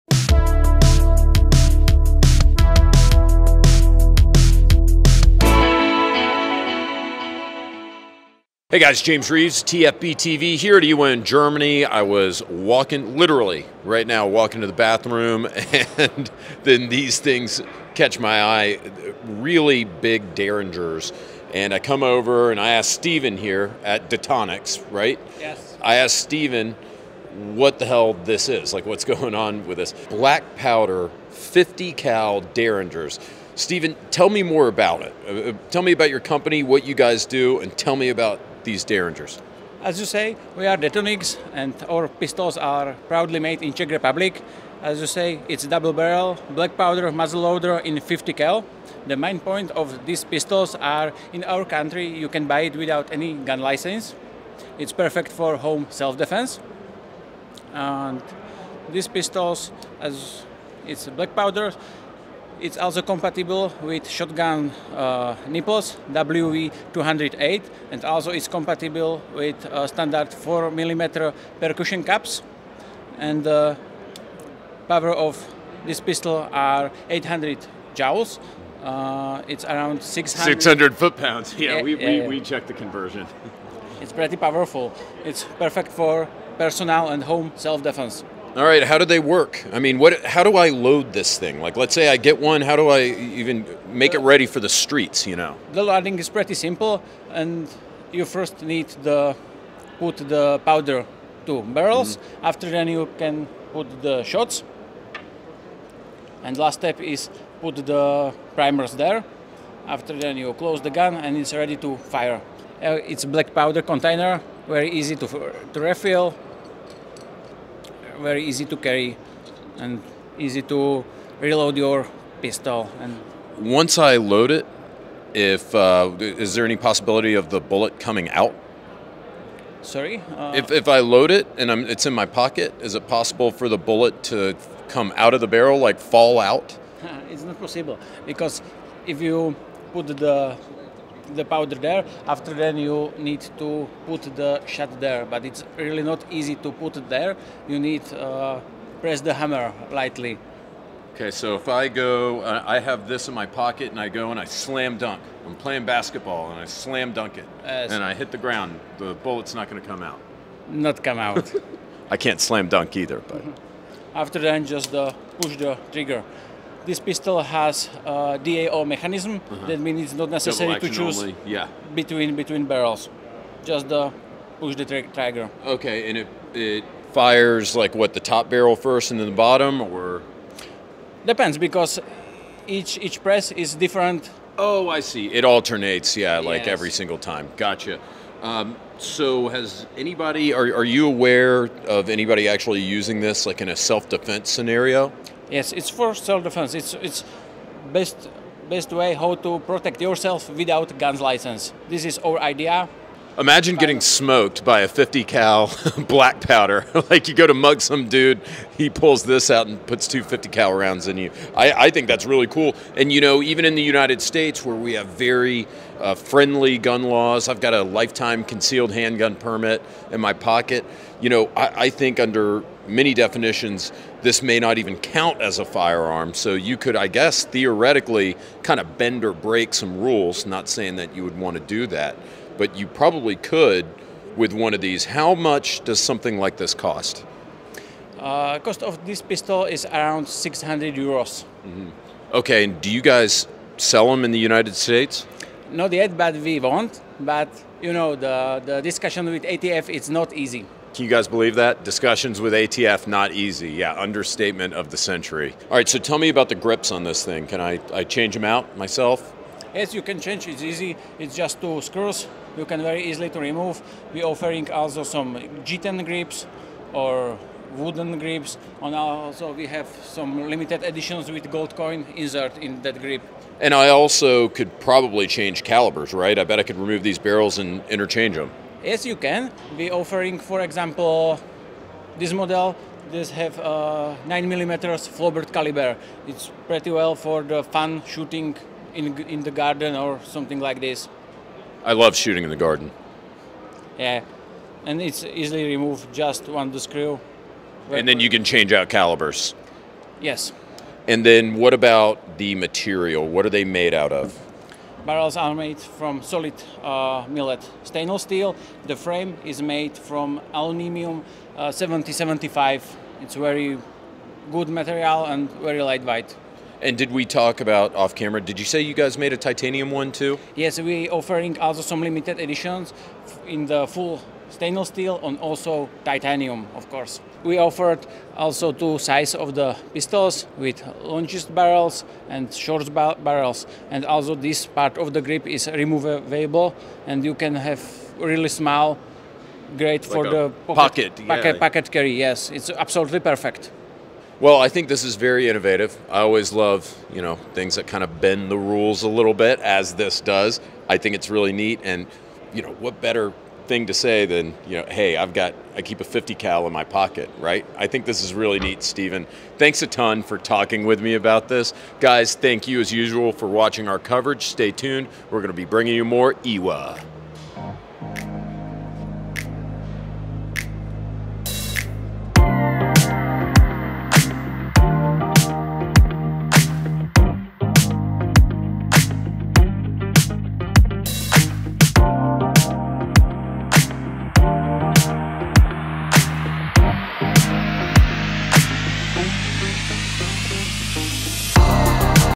A Double Barrel .50 Caliber Pistol?! The Czech Detonics Gladiator [IWA 2022]
In this episode of TFBTV Show Time (don't forget to subscribe), James Reeves is visiting the Czech gun manufacturer, Detonics, to see their Detonics Gladiator, a double barrel .50 caliber derringer that is so small, it can be pocket carried. More importantly, under many jurisdictions it may not even be considered a firearm because it's actually a blackpowder derringer.